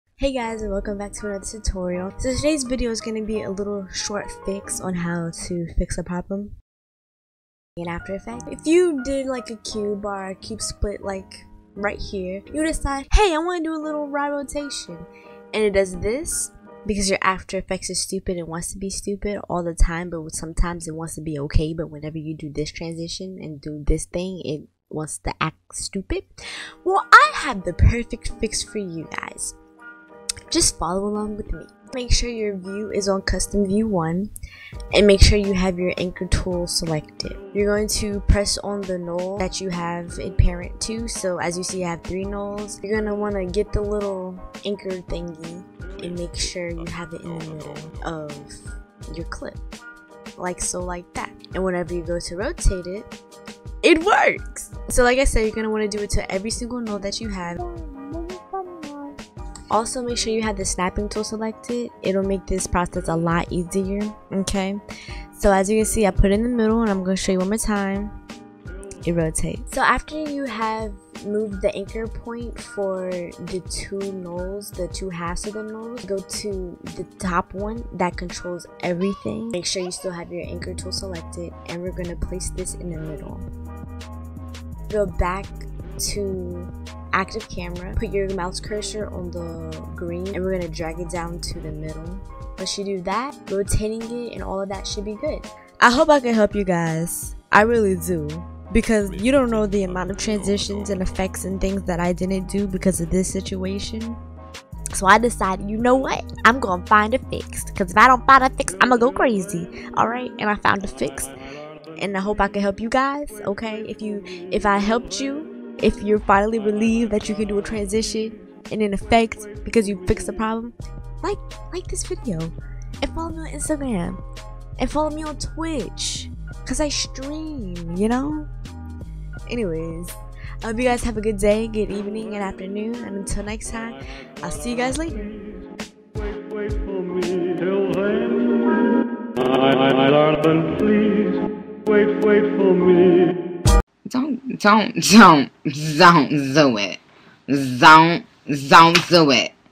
How To Fix Cube Not Rotating Right In Ae
Hii, welcome back to another Monday upload. In todays video I'm showing you how to fix a problem you may be having in after effects with the cube not rotating correctly